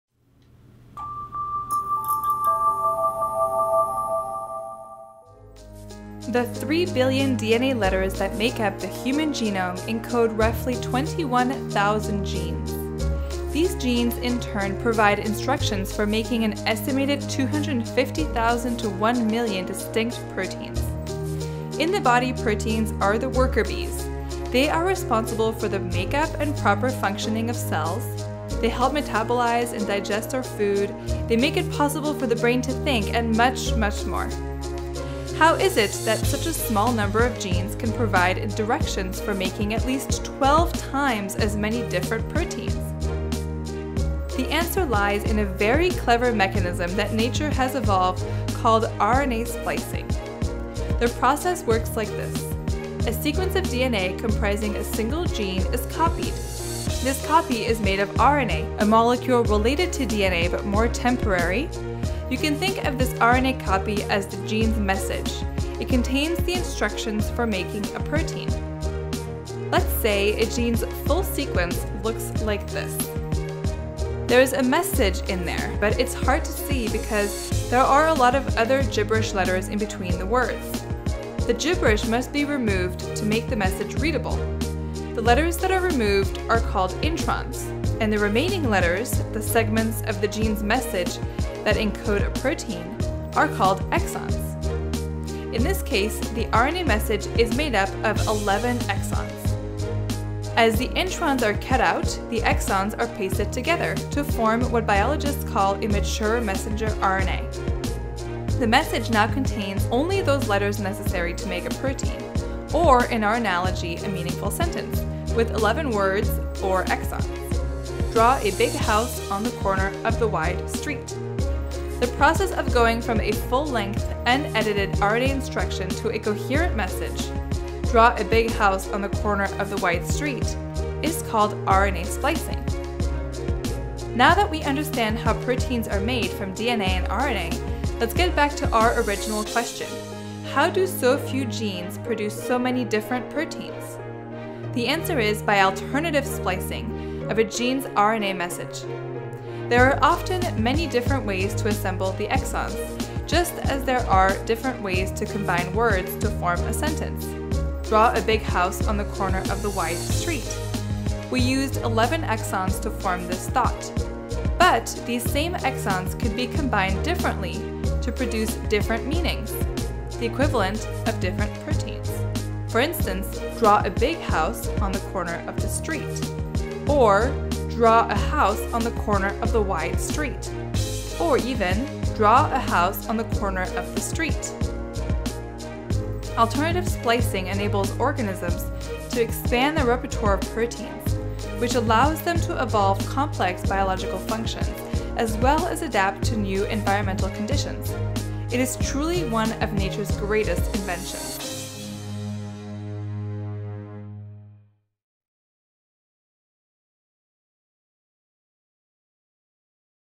Just What is RNA Splicing?
Splicing is truly one of nature's greatest inventions.

YourekaScience and Cold Spring Harbor Laboratory team up to explain how this molecular miracle happens.